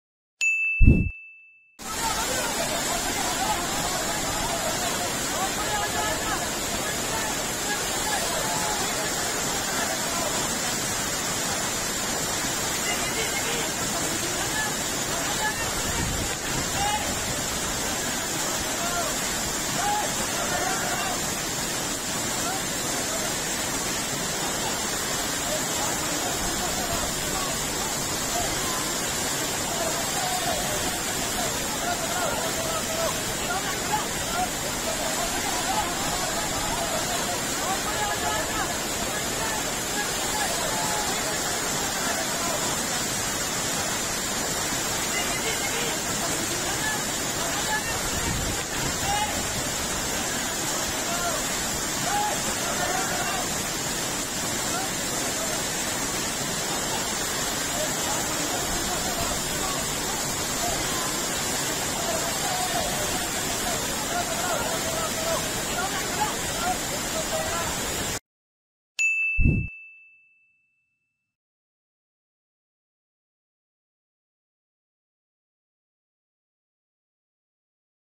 लगातार बारिश और बाढ़ जैसे हालातों ने ली 23 की जान, Nainital में सेना ने किया Rescue Operation
उत्तराखंड में लगातार बारिश से 23 लोगों की मौत हो गई है. राज्य का कुमाऊं क्षेत्र सबसे ज्यादा प्रभावित हुआ है। इस बीच सेना ने नैनीताल से 36 लोगों को बचाया है जो पानी में फंसे हुए थे। यह पता चला है कि अकेले नैनीताल में सोमवार से 400 मिमी से अधिक बारिश हुई है, जिसके परिणामस्वरूप प्रसिद्ध नैनी झील का पानी ओवरफ्लो हो गया है।